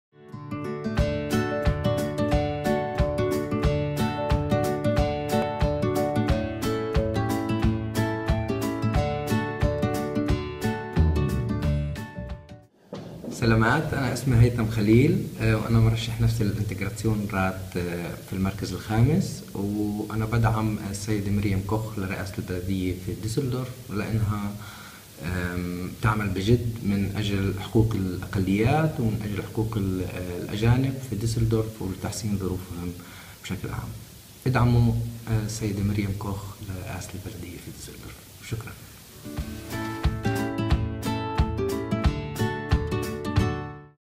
Miriam Koch trifft auf Haitam Khalil
Haitam Khalil ist Kandidat der internationalen offenen GRÜNEN Liste zur Wahl des neuen Integrationsrat. Für unsere arabisch-sprachigen Mitbürger*innen hat er ein kleines Video mit und für Miriam Koch gedreht.